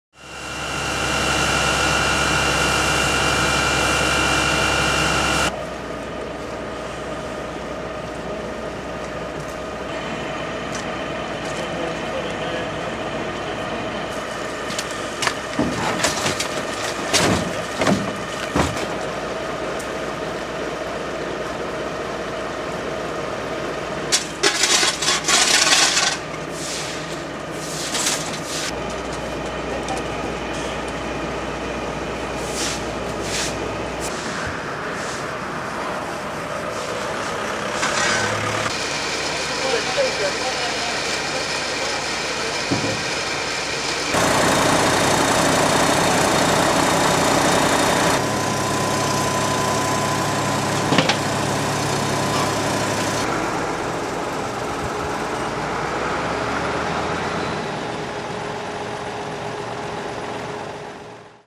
Autobrand Sint Willebrord
Op de parkeerplaats bij de Aldi aan de Dorpsstraat in Sint Willebrord is maandagnacht een auto uitgebrand. De brandweer denkt dat het hier om een pyromaan gaat.